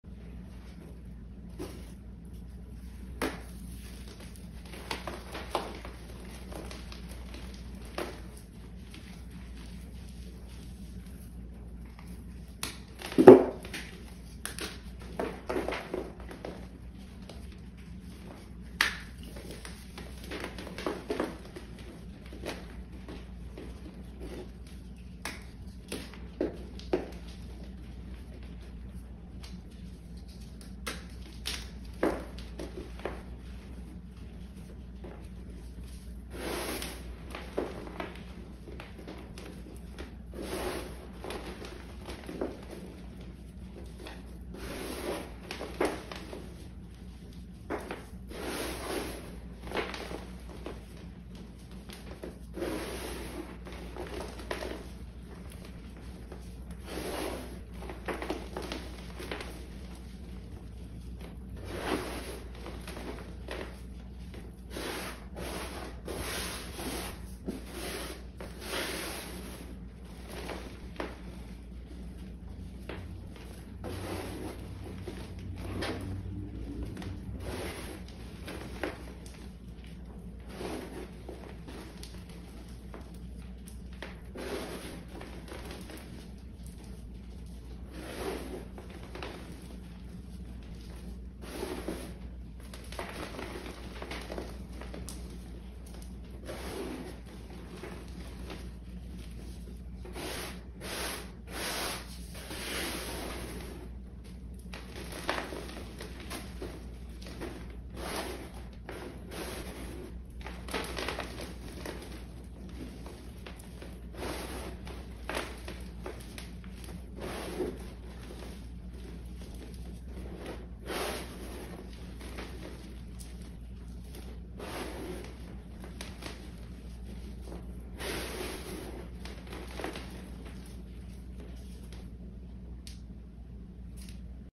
PowderMePinkOctober |Day16| @asmrstudioco M ◼️|Pink Mini Block Reform| Black Chunky ✨
Hey Powder Posse!

(16) PowderMePinkOctober In order to continue to spread breast cancer awareness I am going to do a pink themed or pink incorporated video everyday for the month of October 💗SaveTheBoobies FUCancer BreastCancerAwareness .You can learn more

In this video I have 1 @asmrstudioco M ◼️ w/ a Pink Mini Block Reform & Black Chunky ✨! I combined Black with Brown dyes and the inside was navy blue and turquoise 🤷🏾‍♀️

‼️Please No Edits or Repost To Any Platforms‼️